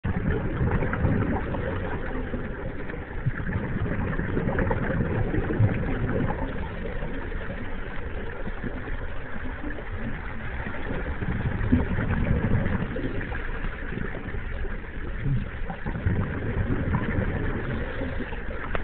scuba at apple gate
Scuba diving at Apple Gate Reservoir, video taken with Casio brigade cell phone